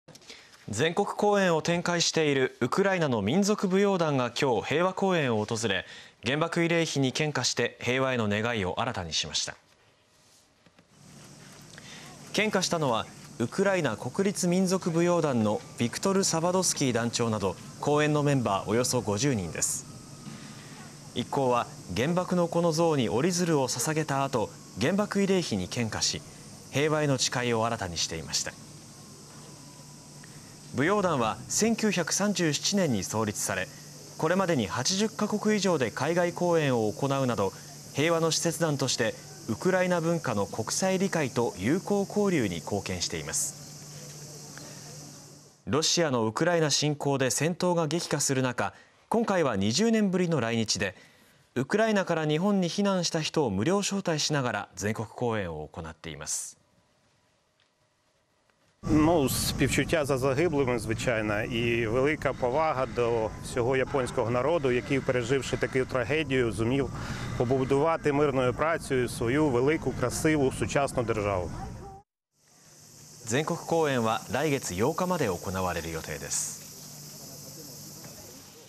ウクライナの舞踏団が平和公園で献花
放送日 2023/7/25　　Aired on July 25, 2023　戦争 平和

Ukrainian Dance Ensemble Offers Flowers at Peace Memorial Park

A Ukrainian dance ensemble touring Japan visited Hiroshima Peace Memorial Park and offered flowers at the Cenotaph for the Atomic Bomb Victims, reaffirming their hopes for peace. Approximately 50 members of the Pavlo Virsky Ukrainian National Folk Dance Ensemble, including Ensemble Leader Victor Zavadsky, offered flowers at the Cenotaph after making an offering of paper cranes at the Children's Peace Monument, reaffirming their commitment to peace. The Ensemble was formed in 1937 and has performed in over 80 countries overseas, contributing to international understanding of Ukrainian culture and friendly relations as peace mission group. As the fighting intensifies in the Russian invasion of Ukraine, the Ensemble, touring Japan for the first time in 20 years, is offering free tickets to Ukrainian refugees in Japan.

"Victor Zavadsky Pavlo Virsky Ukrainian National Folk Dance Ensemble Leader"
I offered flowers here today with a prayer for world peace in my heart. We have been through terrible things,
and it is my hope that these things do not repeat themselves in the future.

The Ensemble will be touring Japan until August 8th.

全国公演を展開しているウクライナの民族舞踊団が、今日、平和公園を訪れ、原爆慰霊碑に献花して平和への願いを新たにしました。献花したのは、ウクライナ国立民族舞踊団のヴィクトル・サヴァドスキー団長など公演のメンバーおよそ５０人です。一行は、原爆の子の像に折り鶴を捧げた後、原爆慰霊碑に献花し、平和への誓いを新たにしていました。舞踊団は１９３７年に創立され、これまでに８０カ国以上で海外公演を行うなど、平和の使節団としてウクライナ文化の国際理解と友好交流に貢献しています。ロシアのウクライナ侵攻で戦闘が激化する中、今回は２０年ぶりに来日でウクライナから日本に避難した人を無料招待しながら全国公演を行っています。

【ウクライナ国立民族舞踊団ヴィクトル・サヴァドスキー団長】
「世界平和への思いを込めて献花しました。すごくつらい思い出があるので、これからもそういうことが起きないよう願っています。」

全国公演は2023年8月８日まで行われる予定です